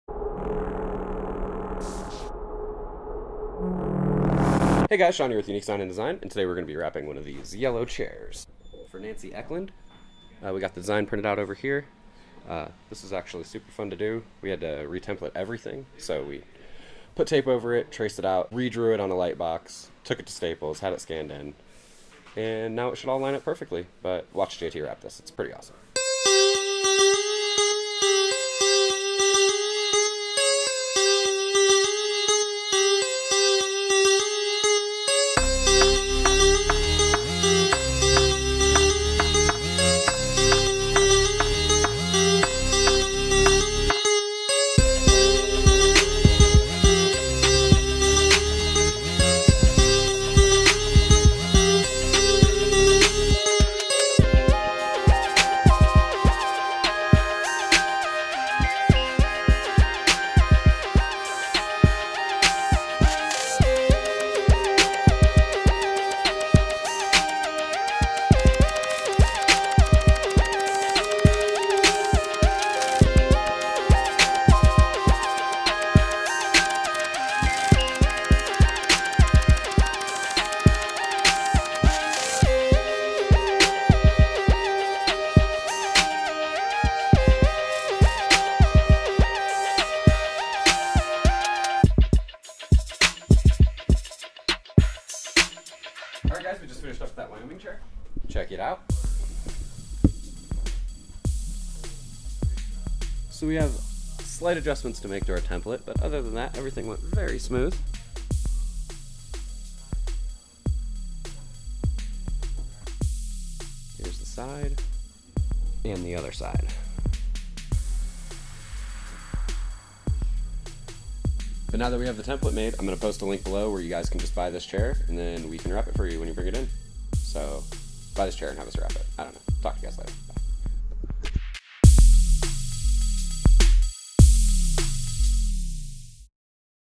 Custom Chair Wrap
This wood Adirondack chair was nice, but our customer had something custom in mind...

Watch us wrap it in templated pieces with the University of Wyoming Cowboys logo and color scheme.

We can use this same template on your chair if you buy the chair at the

Design: Shawn
Install: J.T.
Film&Music: Ohio Snap Images
Host: Shawn